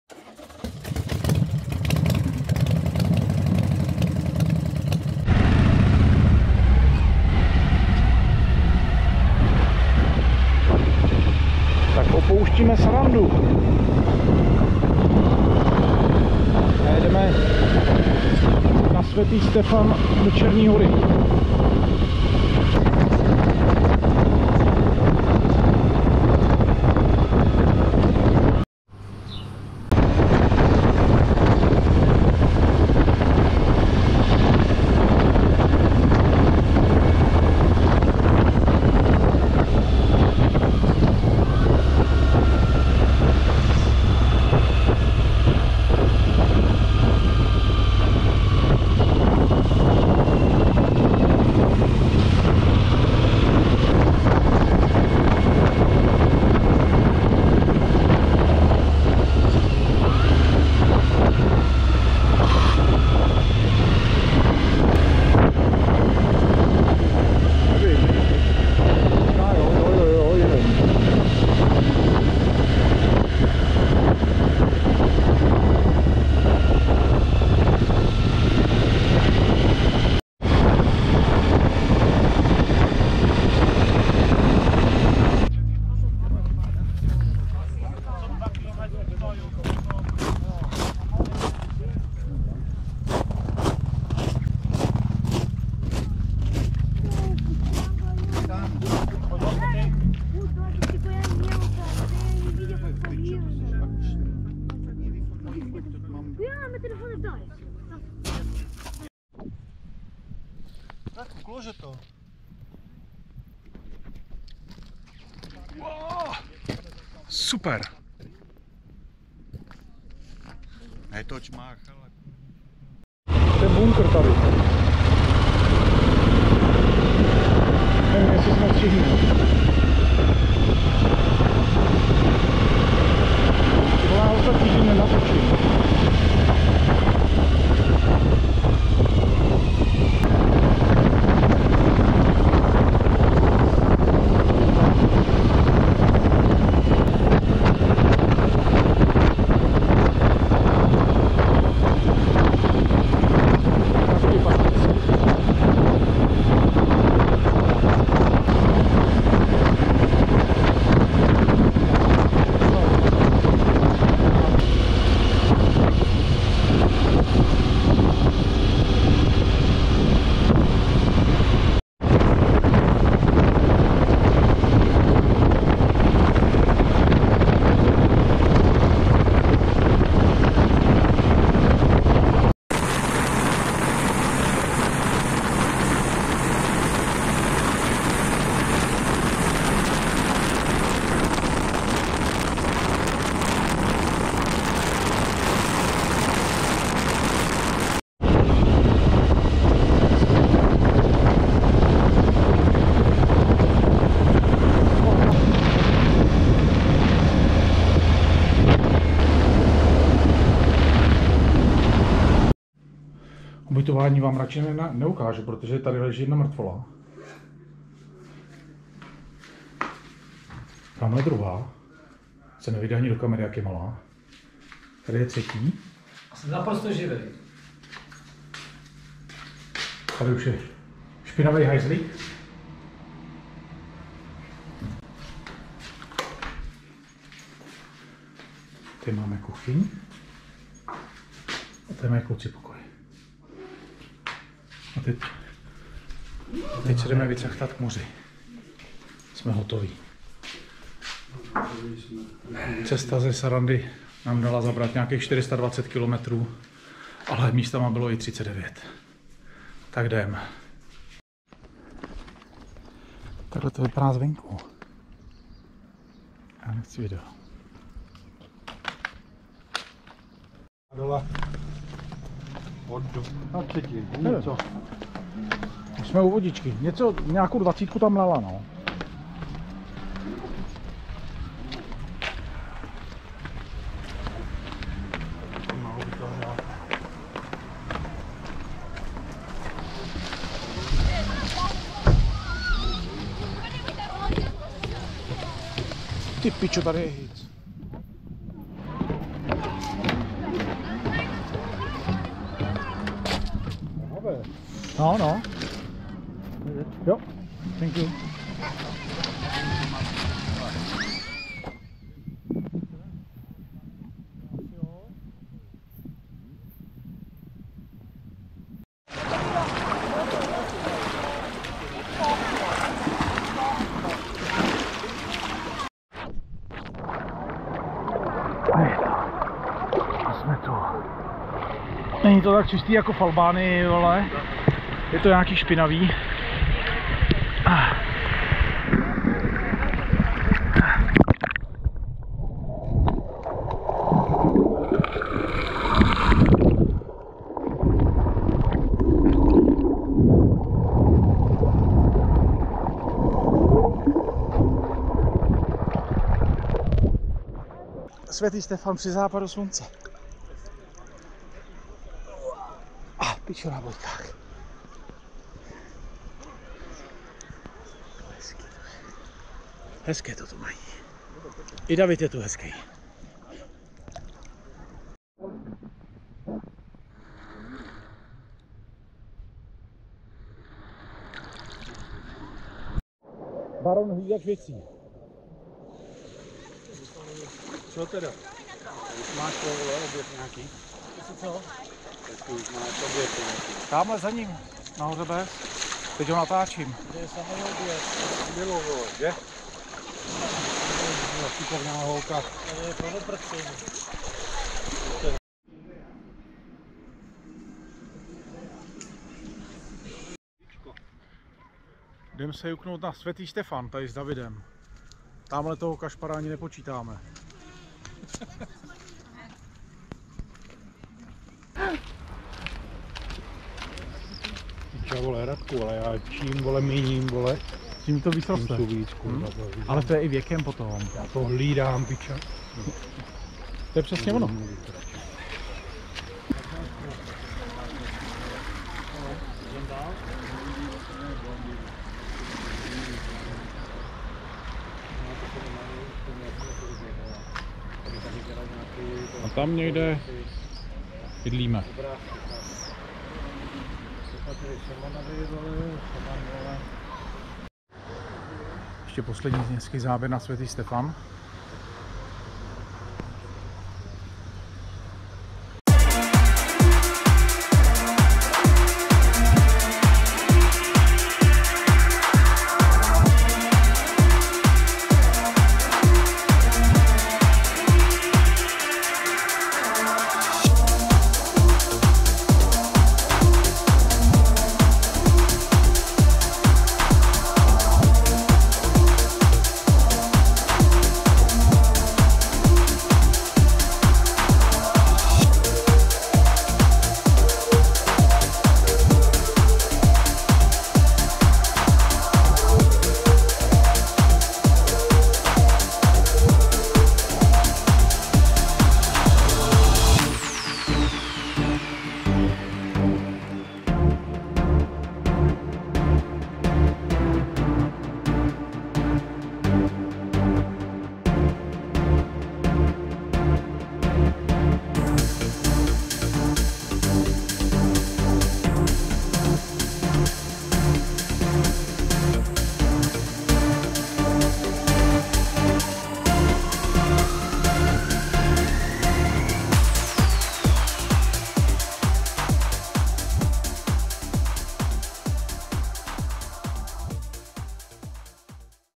Mototrip Albánie 2021 den 7
Dnešní trasa vede ze Sarandë do Svetiho Stafanu v Černé hoře.